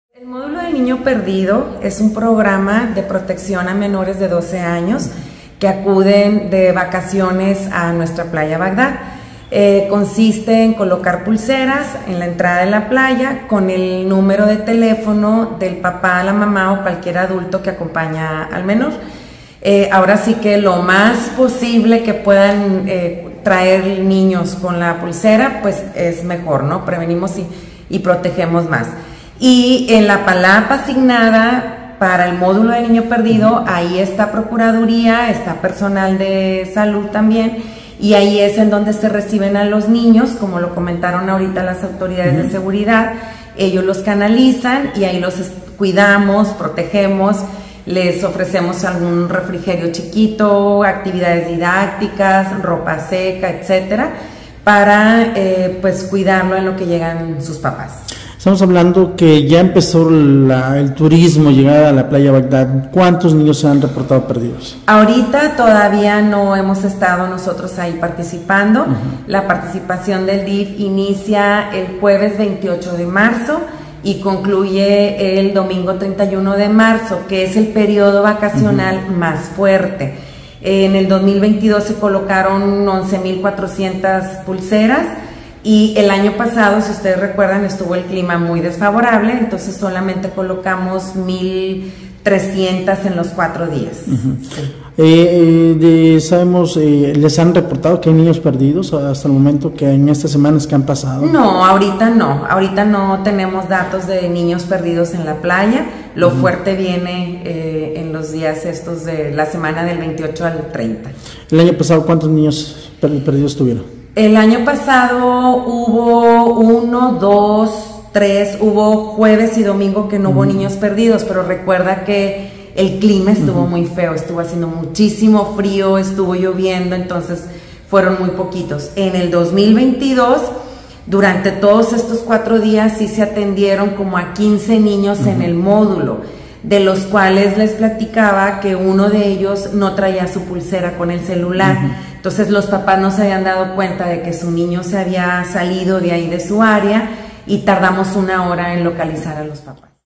SE PREPARAN PARA BRINDAR APOYO A LOS NIÑOS EN LA PLAYA
Cómo cada semana santa el Sistema DIF ya prepara la instalación del módulo del niño perdido.
Aunque cada año proponen que sea módulo de los papás perdidos.